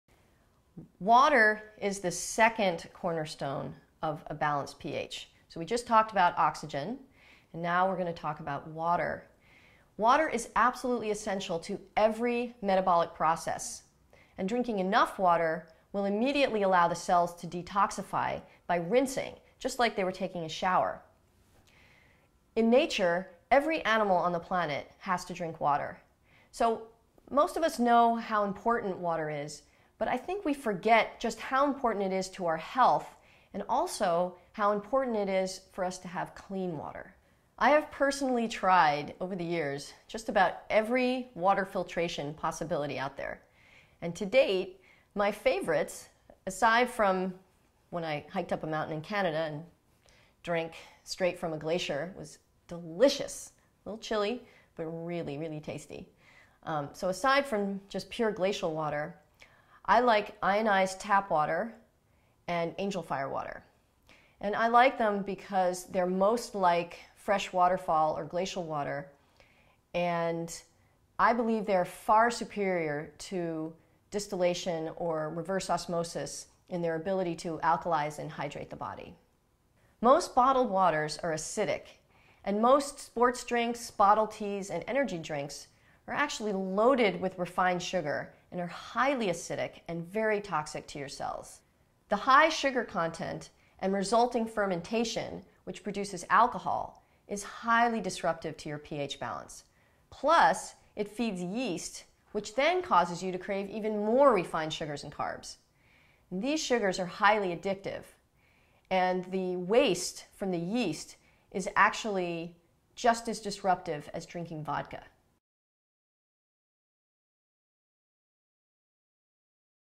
pH Balance: Why is water so important for pH balance?
Karen Atkins shares her tips for maintaining health in today's stressful society, specifically how balancing your pH is one of the key elements for a strong immune system. In this video: How sufficient, quality water helps to balance your pH.